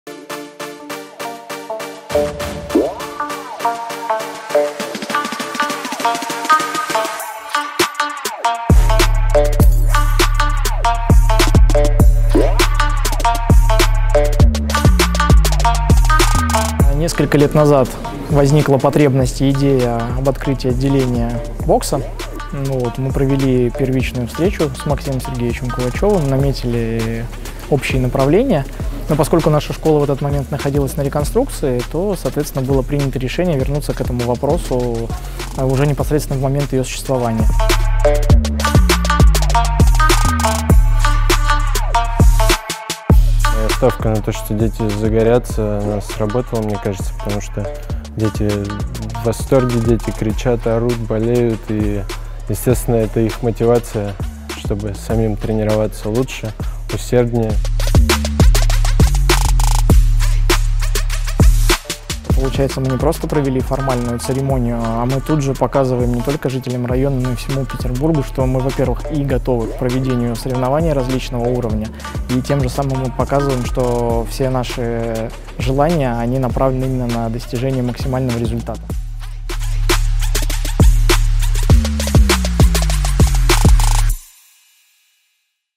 Открытие отделения бокса в ГБУ СШОР Пушкинского района | Спортивная Федерация бокса Санкт-Петербурга
Воскресный день 1 октября ознаменовался важным событием для всего боксерского Петербурга.

В Спортивной школе Пушкинского района открылось отделение бокса. Спустя долгие месяцы плодотворной работы, мы можем с гордостью сообщить, что теперь боксерская карта Петербурга пополнилась новым отделением.

Официальная церемония открытия состоялась в присутствии почетных гостей: главы Пушкинского района Санкт-Петербурга Андрея Чапурова, заместитель председателя Комитета по физической культуре и спорту Санкт-Петербурга Елены Павловой и заместителя руководителя Спортивной Федерации бокса Санкт-Петербурга Максима Кувачева.

В честь открытия отделения, в стенах спортивной школы прошел открытый ринг для юных боксеров Северной столицы.